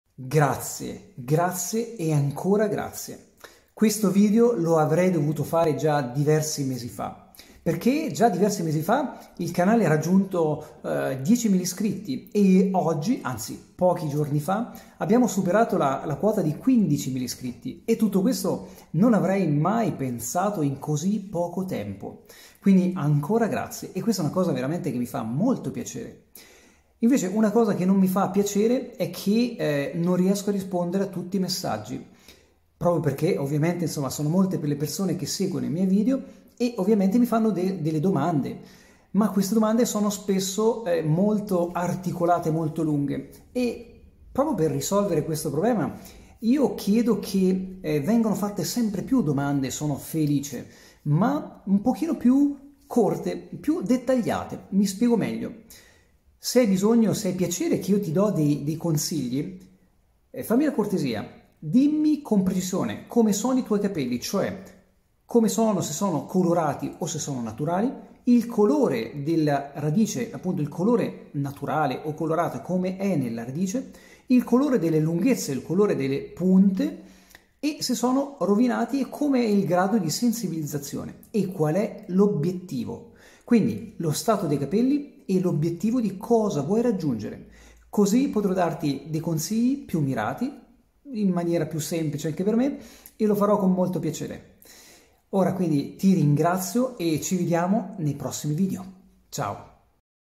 Quota 1️⃣5️⃣.0️⃣0️⃣0️⃣🎊, GRAZIE GRAZIE GRAZIE🥰 Gli Appassionati dei capelli, Sempre più Numerosi😱
Questo breve video è dedicato al ringraziamento dei miei iscritti al canale Appassionati dei capelli

Circa un anno fa, con dubbio e perplessità ho pensato che sarebbe stato giusto e soprattutto utile iniziare a contribuire e divulgare le mie esperienze di 32 anni di lavoro Come acconciatore Parrucchiere, Lo fatto con il cuore e con enorme fatica visto gli enormi impegni che tutti noi professionisti Parrucchieri abbiamo ,tuttavia con  questo ancora piccolo traguardo, e con  tutti i messaggi e contatti che ricevo via email e sotto i miei video, mi da la spinta ed una grande soddisfazione a continuare e cercare di colmare sempre più dubbi e insegnare esperienze per chi ha la passione nel cuore e la vuole mettere in pratica .

GRAZIE GRAZIE GRAZIE

Ciaoooooo